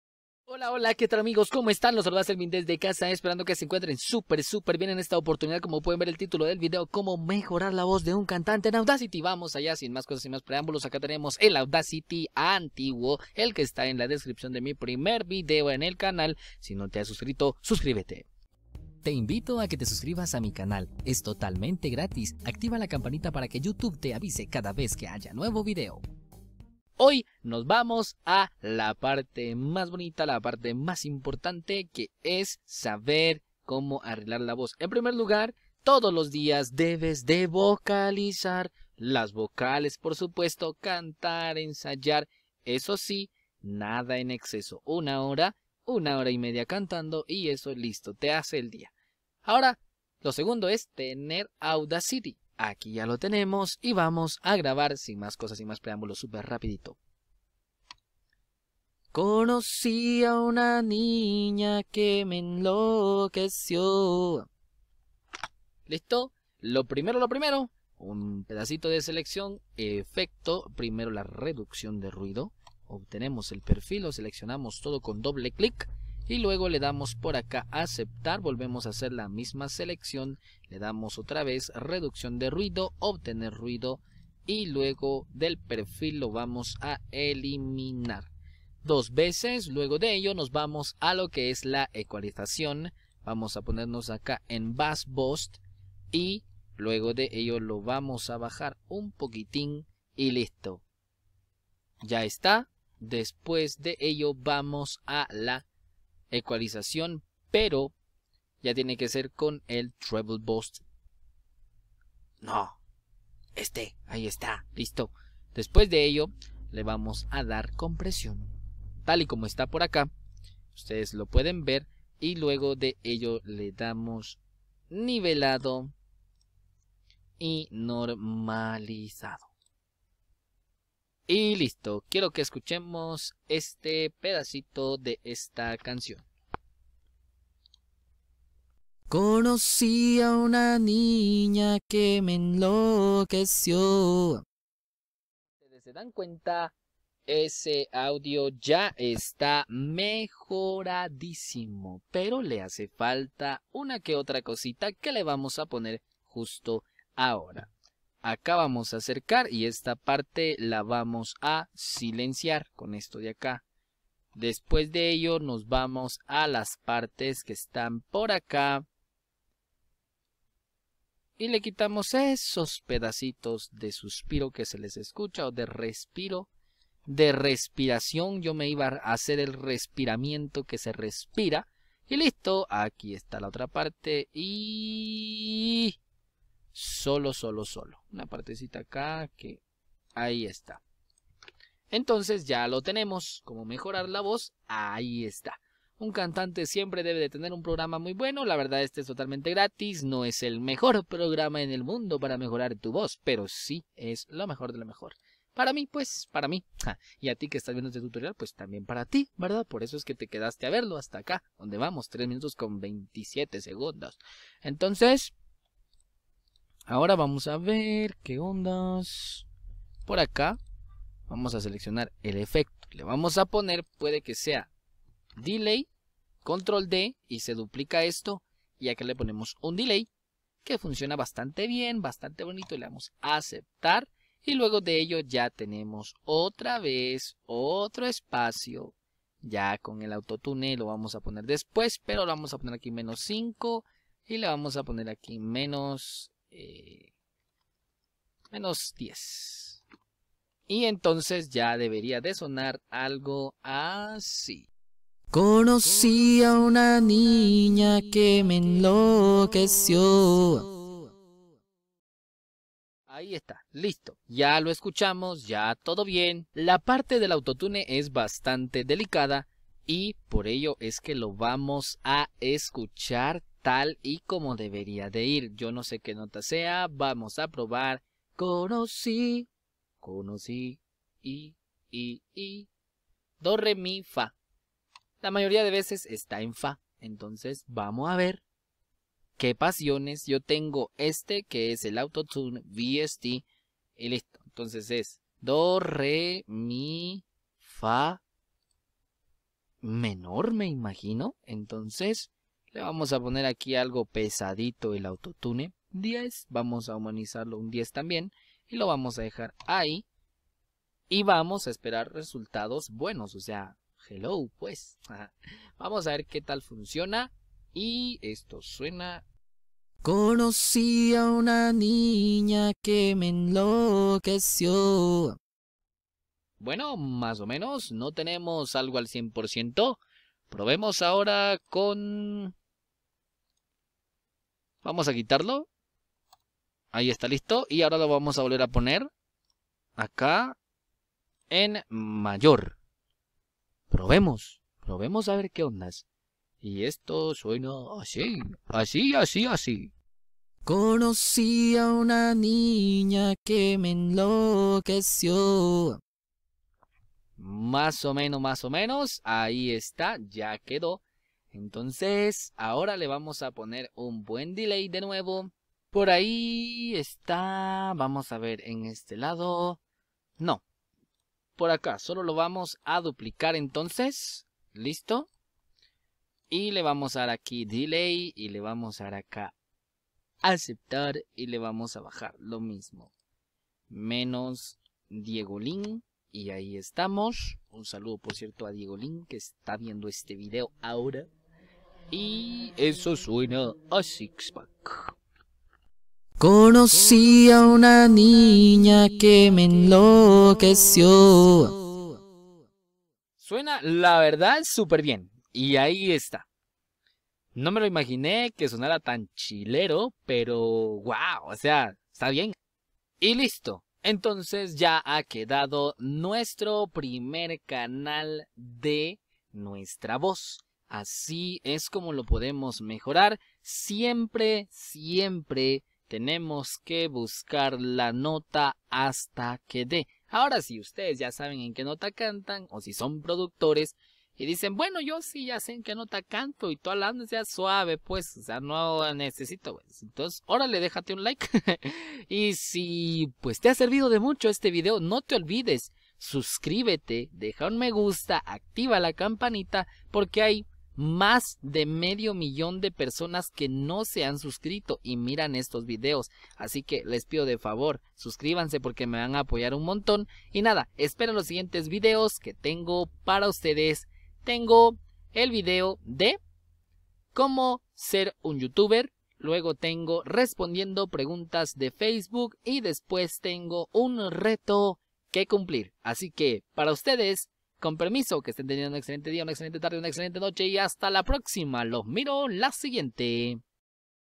Cómo MEJORAR la VOZ de un CANTANTE en Audacity con AUTO TUNE
Espero os guste este vídeo con la nueva técnica de mejoramiento de voces.
Más adelante haré otro tutorial...
A quienes comenten les dejaré un saludo al final de mi siguiente vídeo...

Sígueme y te sigo :v